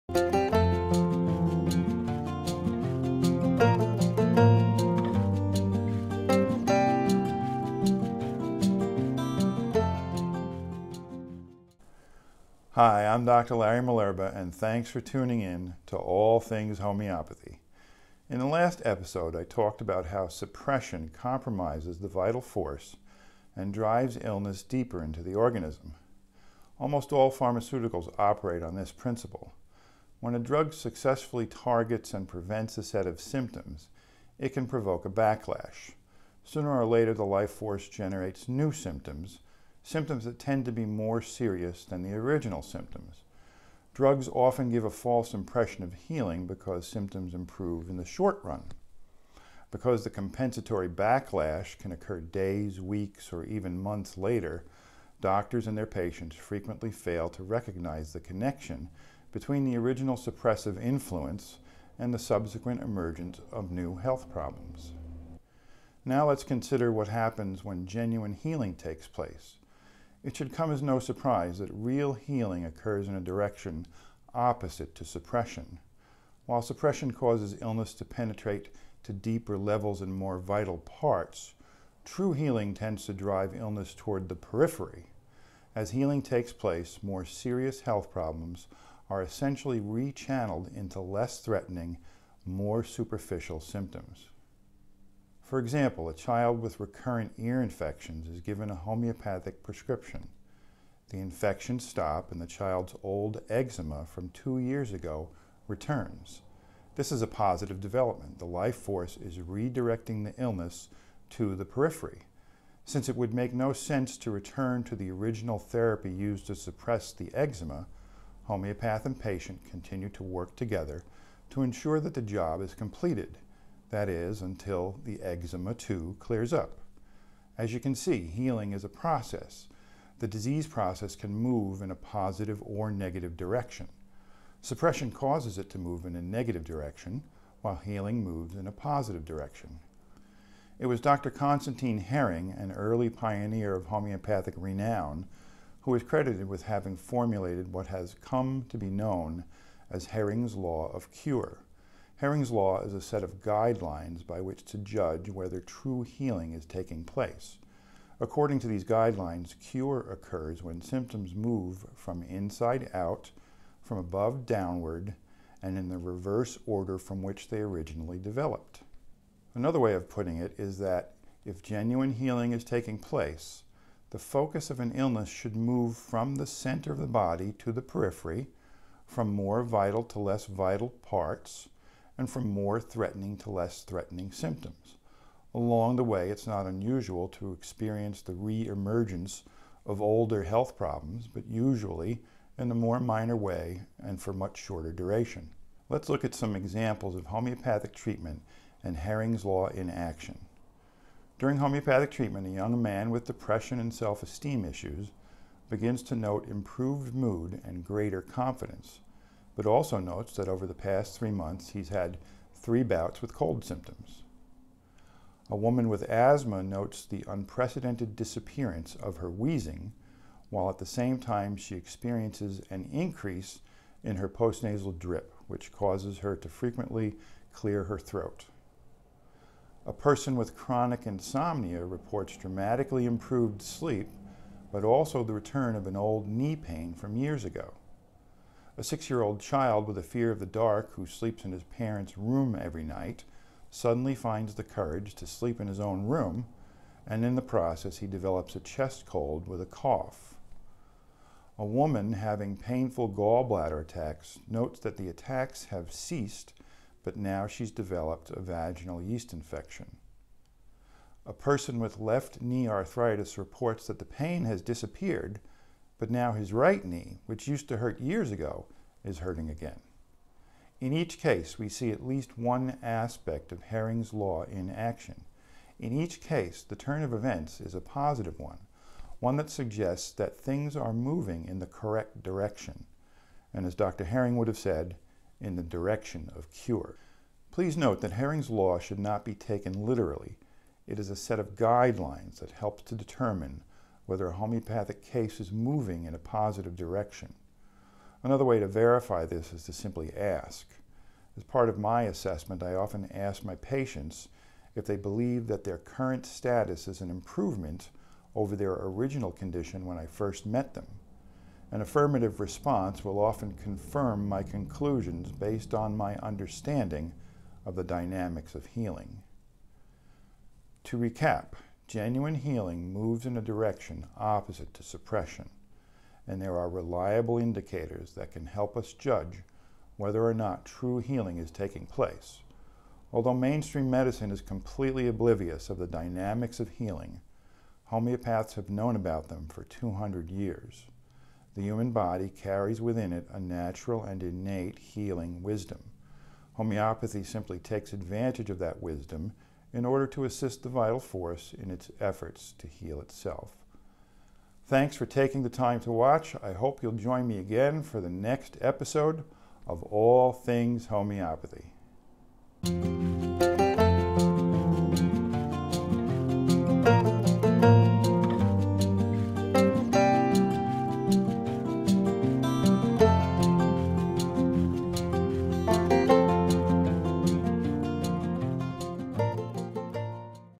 ATH #11: Direction of Cure (Homeopathic healing is not just random)
In Episode 11 of the All Things Homeopathy series, Dr Larry Malerba discusses the direction of cure. Real healing moves in a direction opposite to suppression. Hering's Law can serve as a guide to healing. Dr. Larry Malerba is building this video library as his contribution to the world because he believes that homeopathy is safe, effective, and has the power to heal illness, and the ability to transform human consciousness. It is intended for everyone, including beginners and those with homeopathic experience. May the Vital Force be with you!